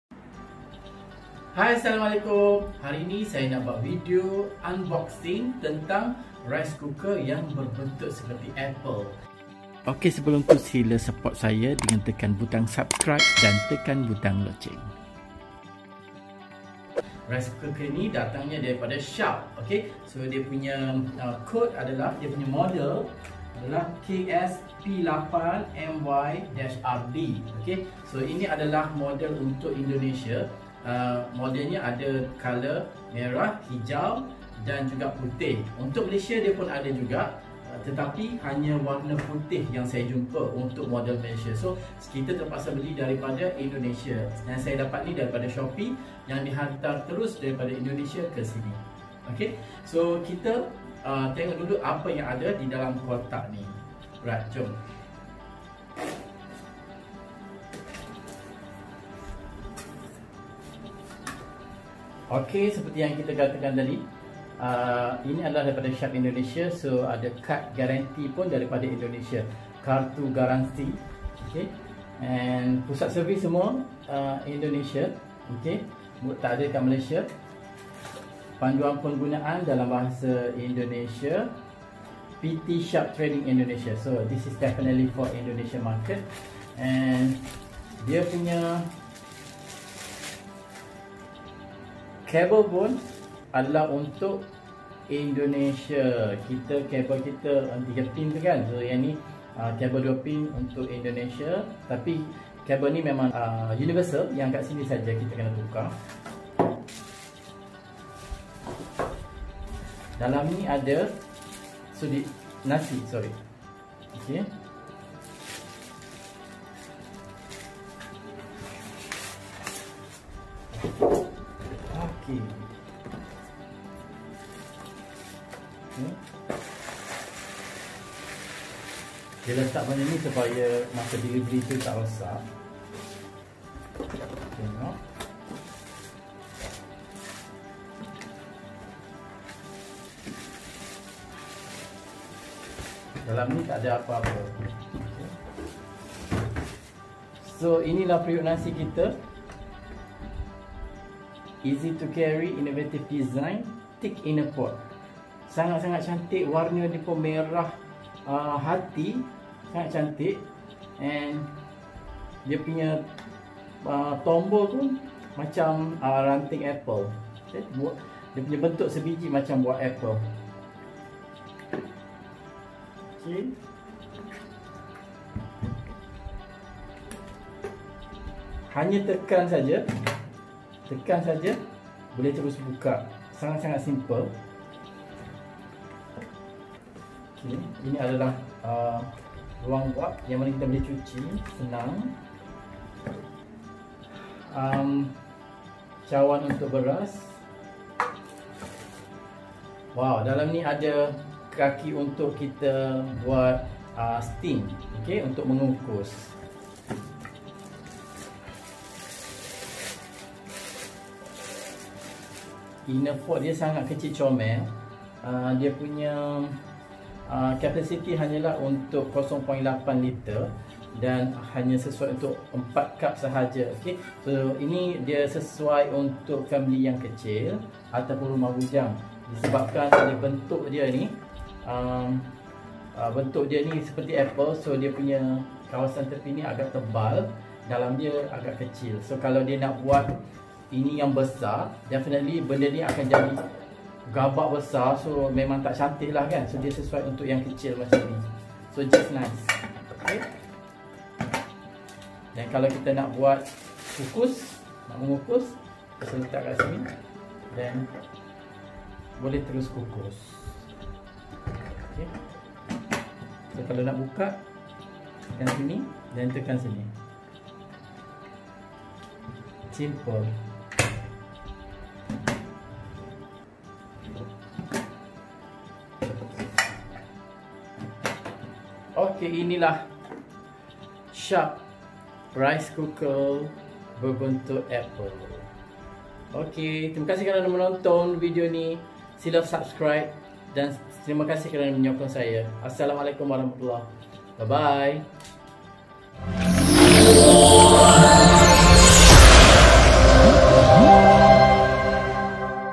UNBOXING | SHARP Apple Rice Cooker
There are two different model number for Malaysia and Indonesia. Malaysia so far only has white colour. Indonesia version has Red, White and Green. I ordered this from Shopee Indonesia.

KSP8MVWH (MALAYSIA)
KSP8MYRD (INDONESIA)